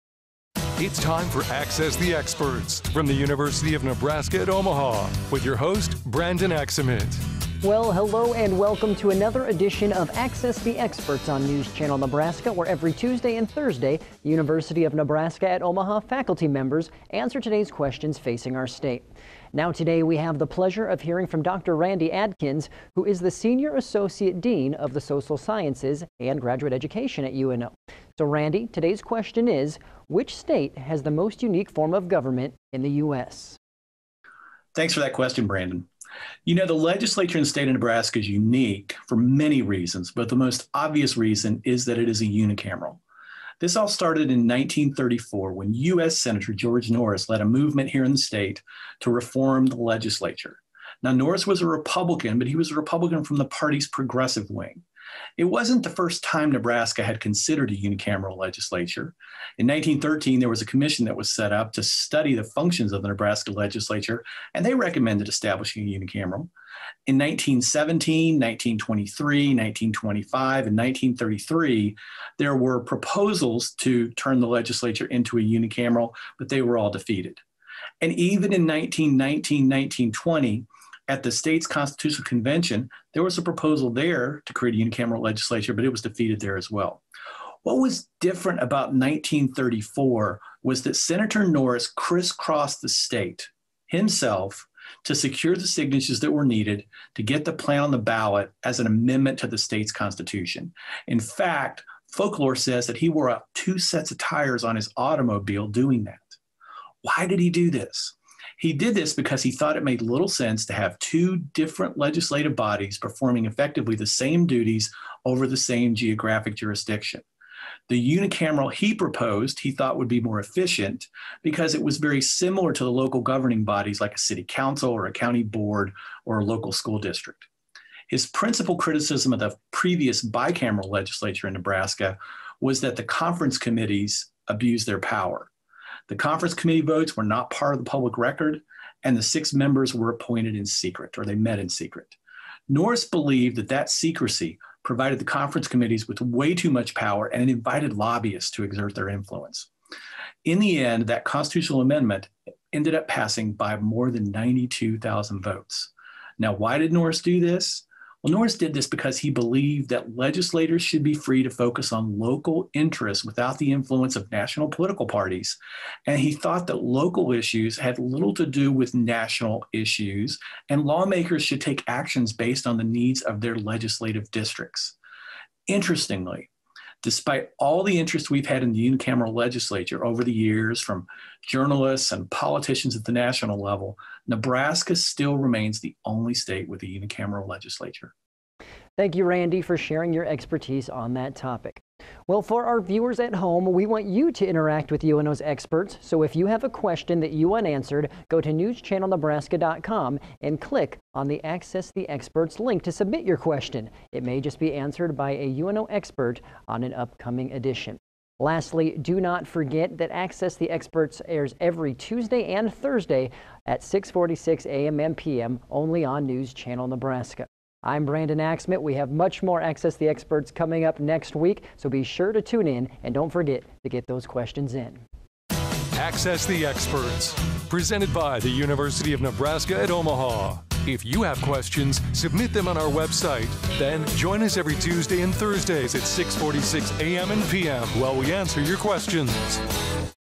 Access the Experts: Dr. Randy Adkins, UNO Senior Associate Dean of Social Sciences & Grad Education
In this installment of Access the Experts: Dr. Randy Adkins, UNO Senior Associate Dean of the Social Sciences and Graduate Education, answers the question: “Which state has the most unique form of government in the US?”